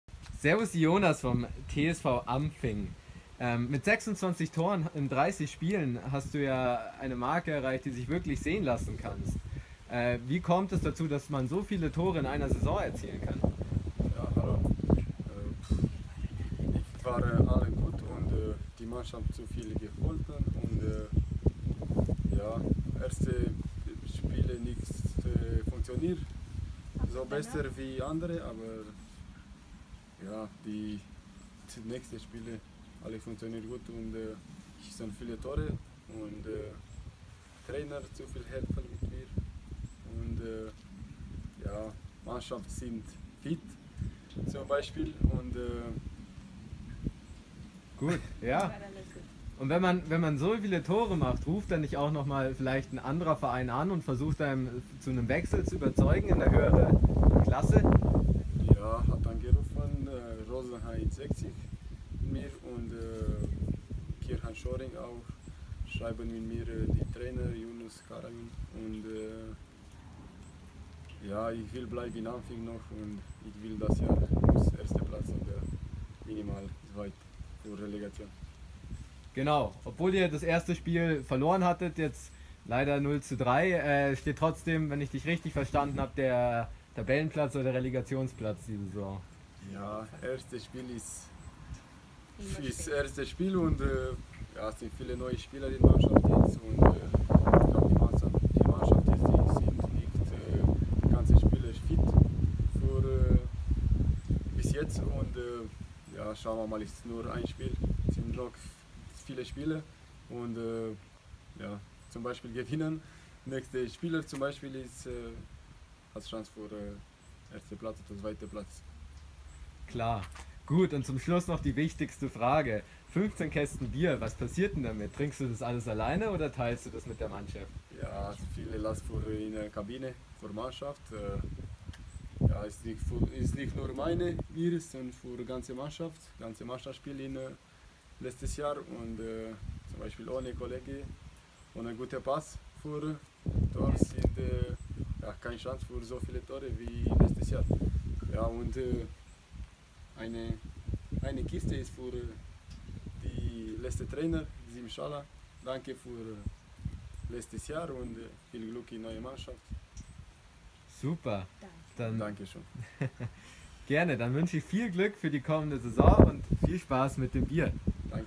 Torjäger Ionas Alin vom TSV Ampfing im Interview
Zurecht stehen nun 15 Kästen ERDINGER in der Garage von Ionas Alin. Mit 30 Toren in 26 Spielen krönte sich der Stürmer vom TSV Ampfing zum Torschützenkönig der Bezirksliga Ost. Im Interview mit Fussball Vorort spricht der 26-Jährige über seine Ziele mit Ampfing und über Angebote aus höheren Ligen.